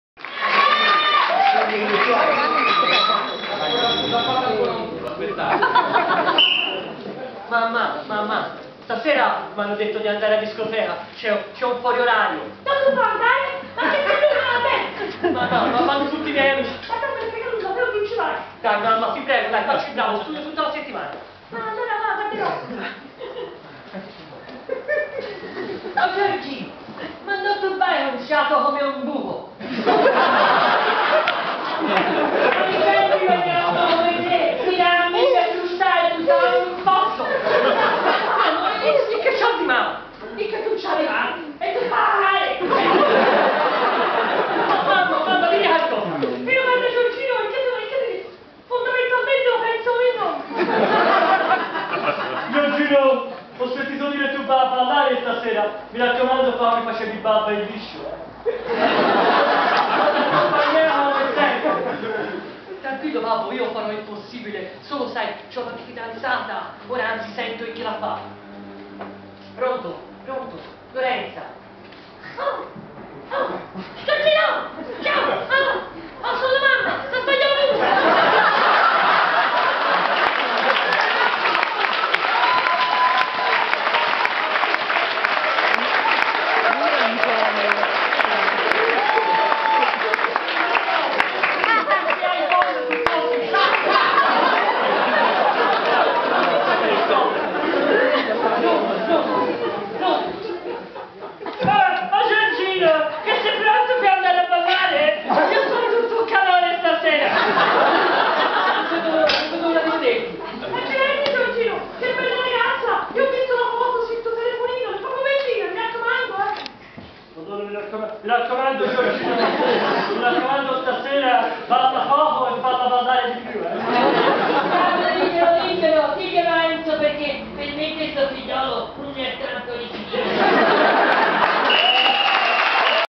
One Man Show Neri_7ott.AVI
Seconda Giornata del campionato amatori del Match di Improvvisazione Teatrale - L.I.F. Lega Improvvisazione Firenze